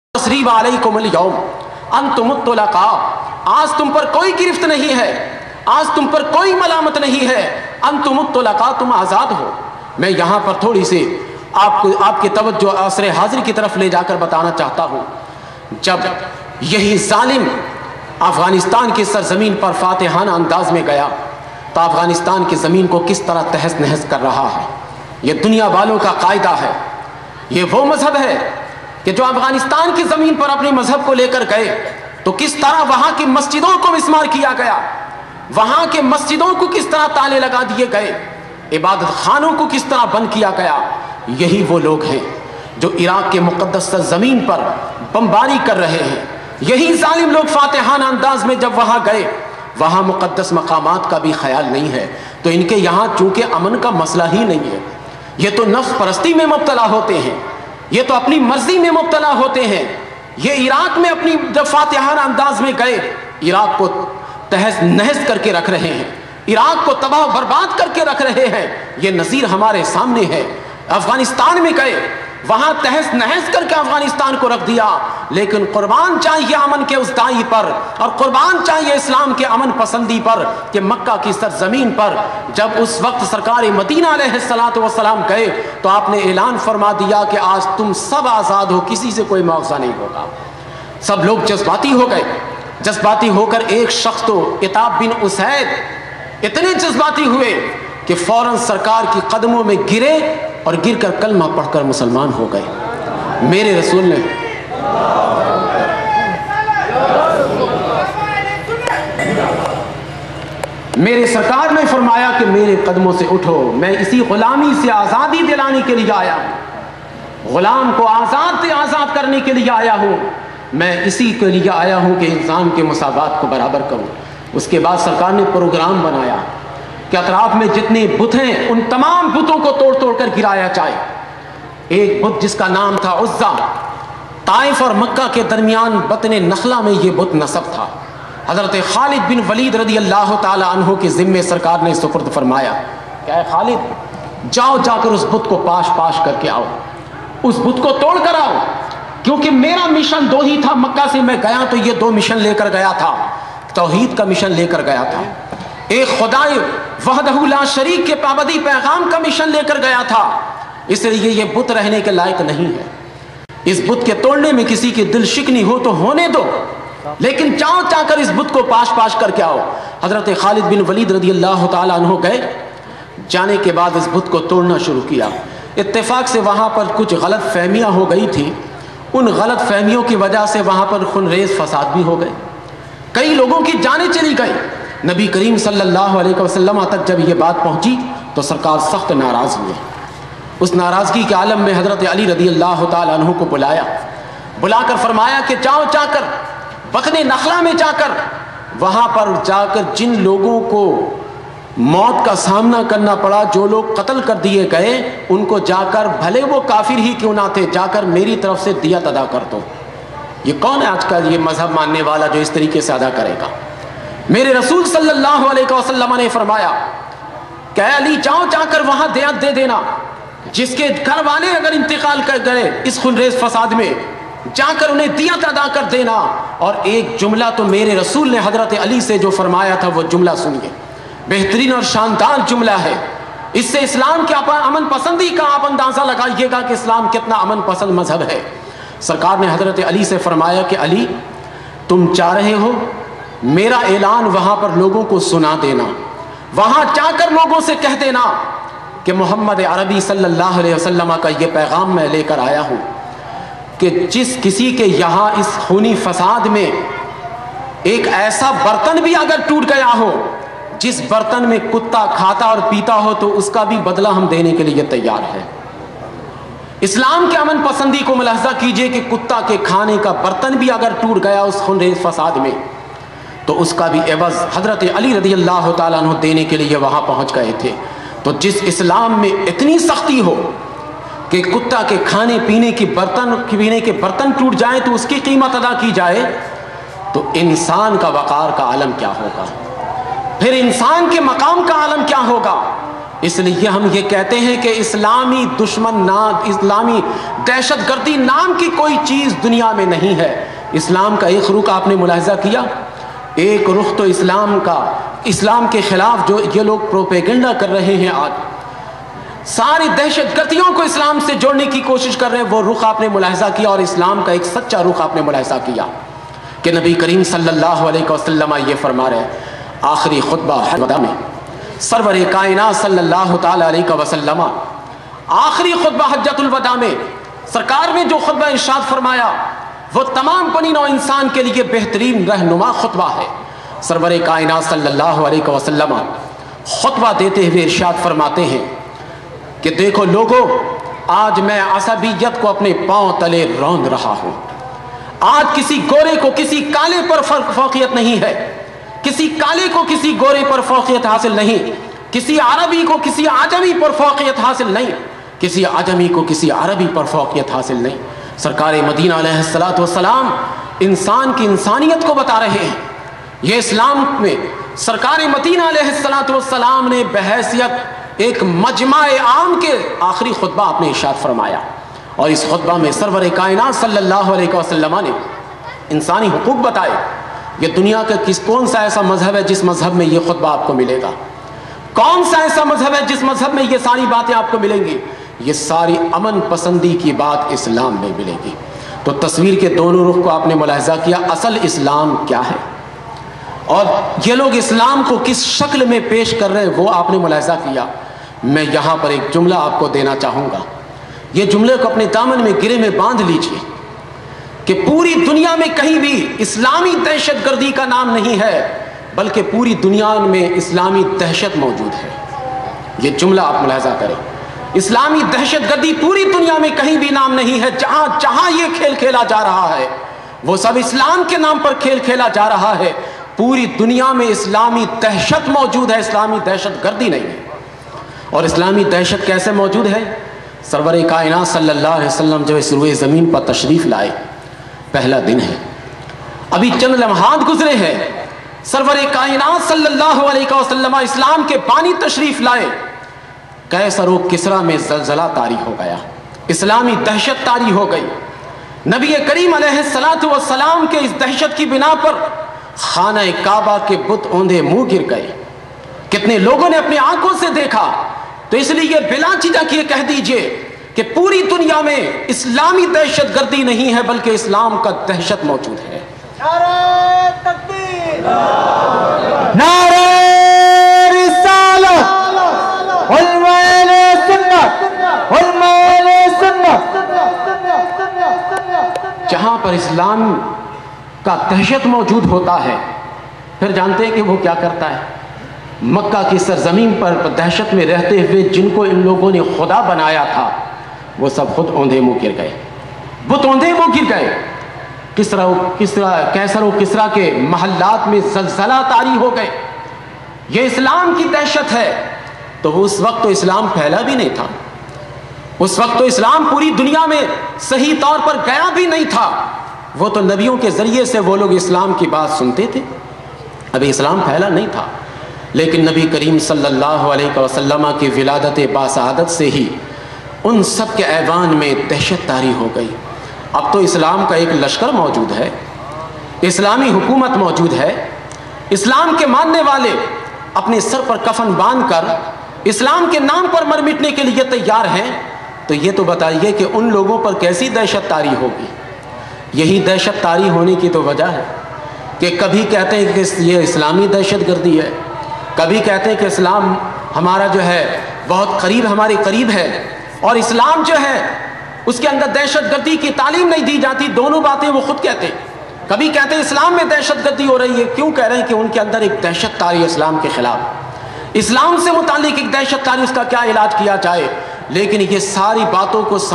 Part-6, Islam aur dehshat gardi
Khateeb ahly sunnat ALLAMA-O-MOLANA SYED SHAH MUHAMMAD  MUMTAZ ASHRAFI (Jama-e-Masjid Aqsa)..